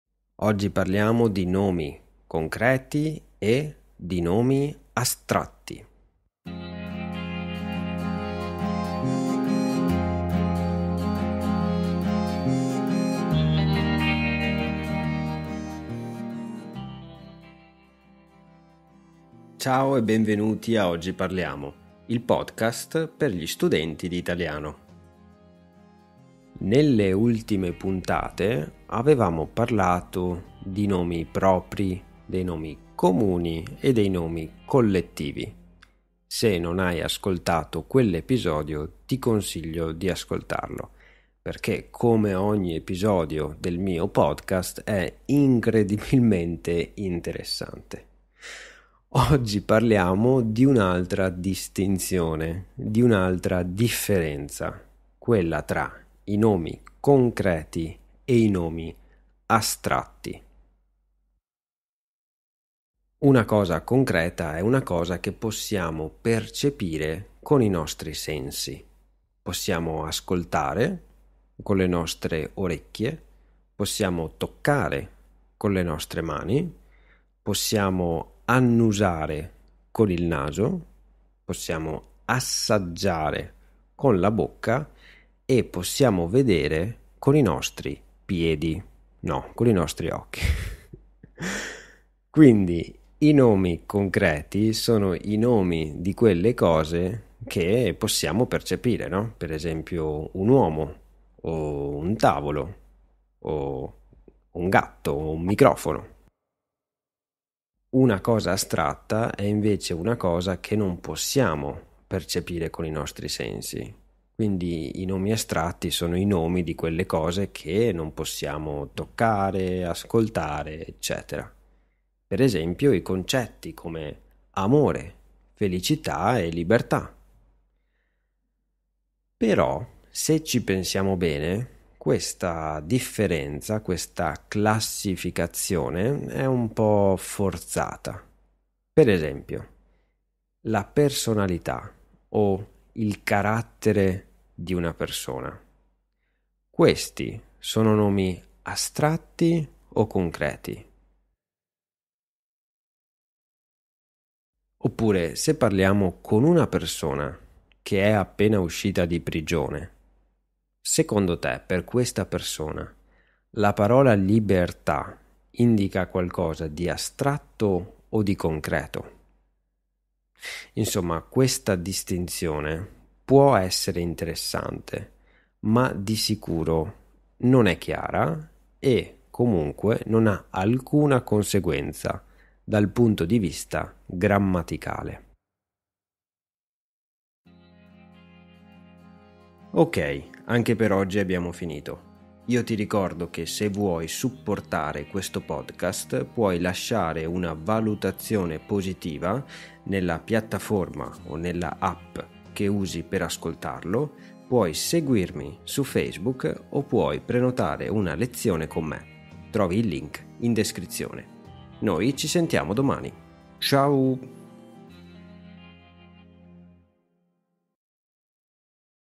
GRAMMATICA: Nomi concreti e astratti
Oggi parliamo dei nomi concreti e dei nomi astratti: che cosa sono?